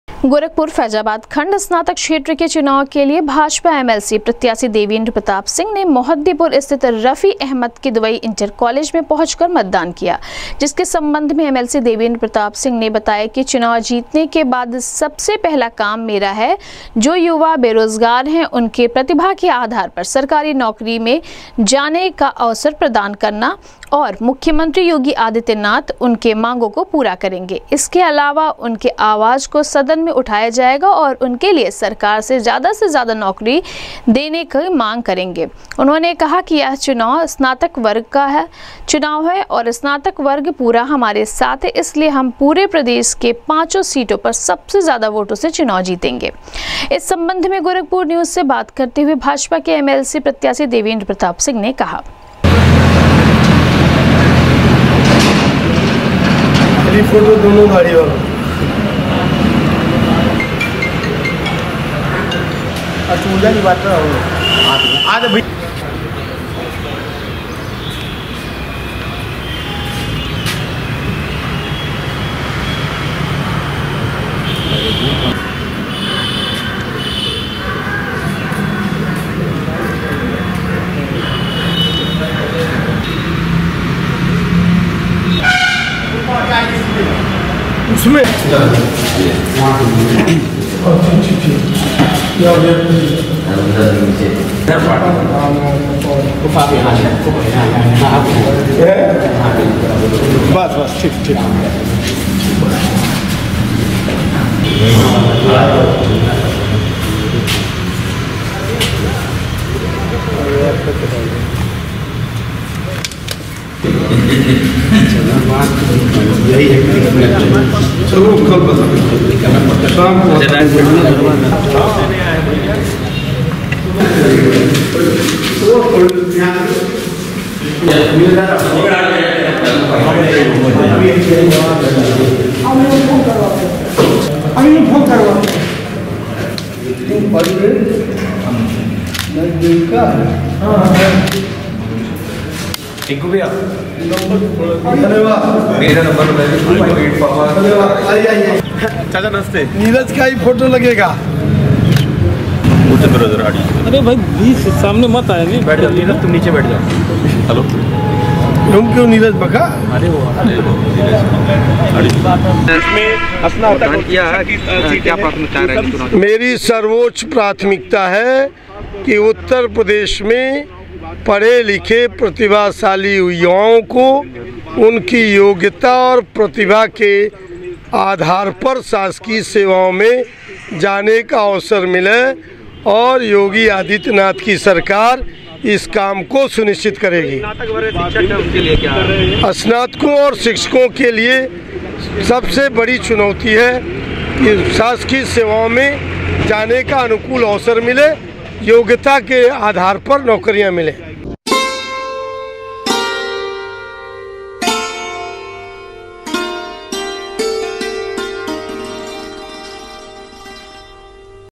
भाजपा के एमएलसी प्रत्याशी देवेंद्र प्रताप सिंह ने मोहद्दीपुर स्थित रफी अहमद किदवई इंटर कॉलेज में
भाजपा के एमएलसी प्रत्याशी देवेंद्र प्रताप सिंह ने मोहद्दीपुर स्थित रफी अहमद किदवई इंटर कॉलेज में पहुँचकर किया मतदान,कहा,चुनाव जीतने के बाद प्रदेश के प्रतिभाशाली युवाओं को उनके योग्यता के आधार पर नौकरी दिलाने का किया जायेगा कार्य